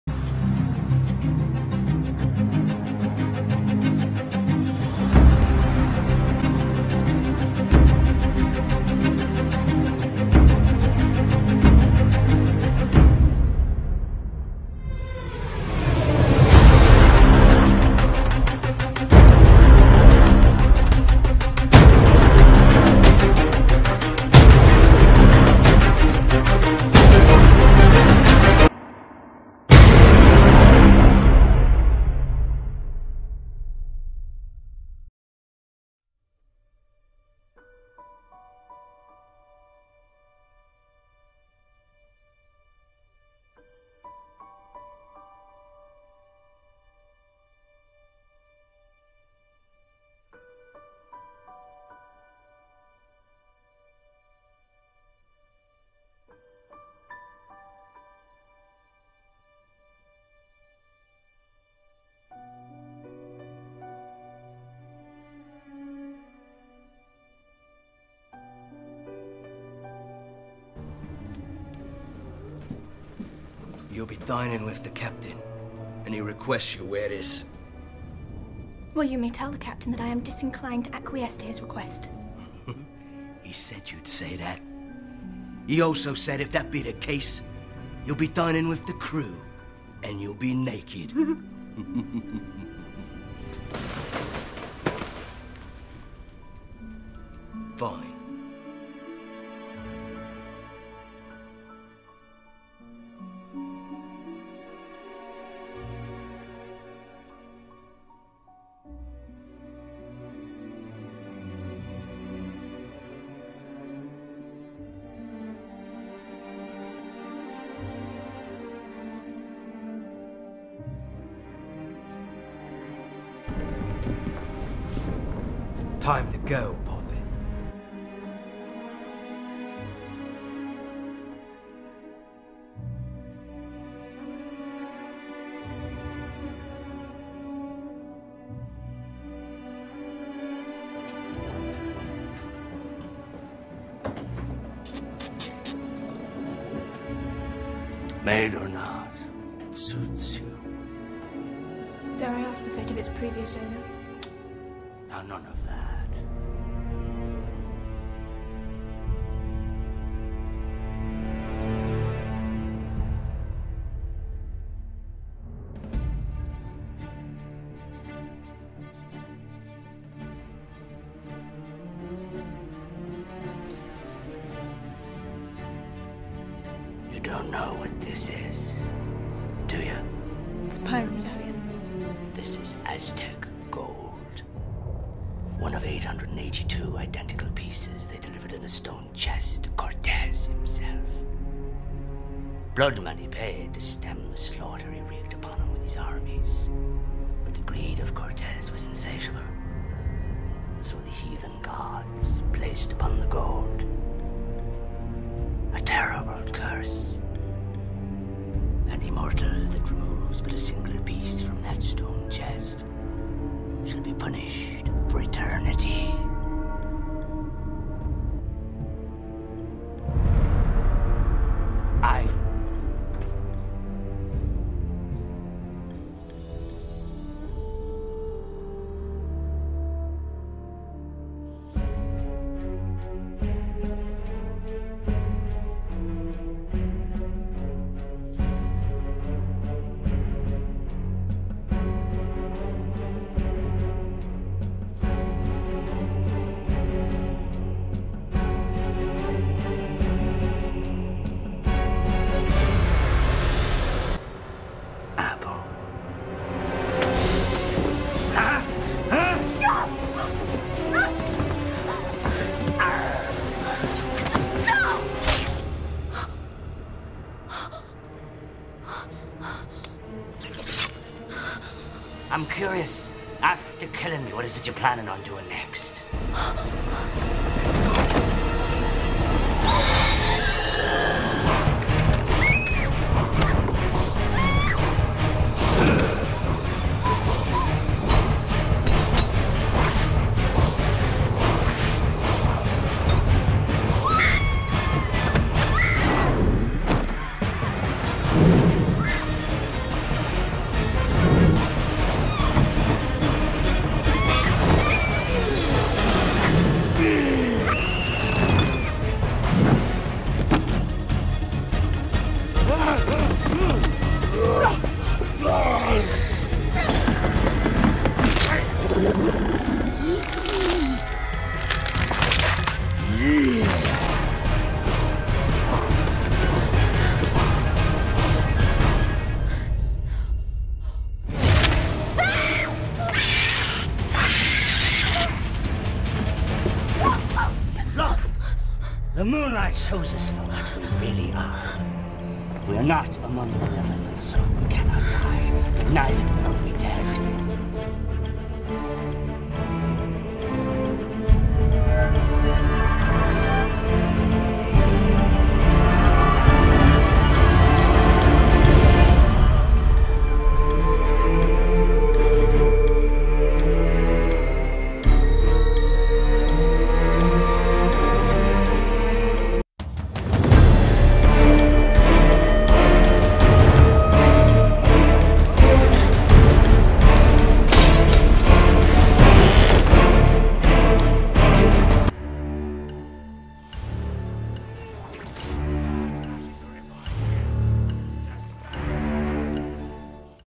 Love Freedom Episode 4 (100+ subs dedication) - Reveal of old scars
Hey, everybody! Sorry it took so much for this to come out! School's been mad the last couple of weeks...
So, this is the 4th part of my movie-series! I would like to dedicate this to all of my woderfull subscribers, without which I may not have been here!

Note: at the point where you see the Evenstar, just pretend it's the pirate medallion, falling from the sleeve of the dress! :)

~Plot~
Elizabeth is aboard the pirate ship. A dress is brought to her. From the sleeve, falls a medallion with two initials carved on it. At dinner, she questions Barbossa about this. He tells her the story of the medallions, that were a symbol of loyalty to that crew! When Barbossa declares that they may very well kill her, she attacks him. She gets to see the walking skeletons, but when they were close to her, with their swords out, an unknown rescuer(Will :) ) gets her out of there.
Elizabeth retreats into her room, hearing whip lashes from above.